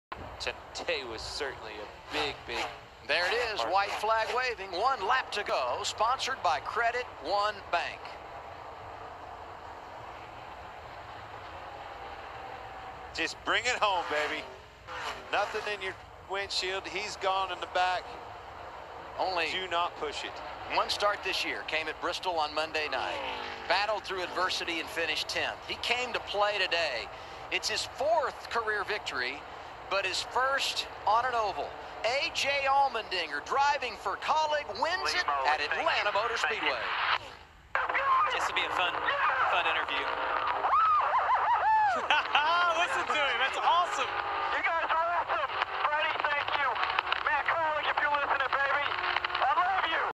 A.J Allmendiger wins the 2020 EchoPark 250 at Atlanta
ALL CREDIT GOES
NASCAR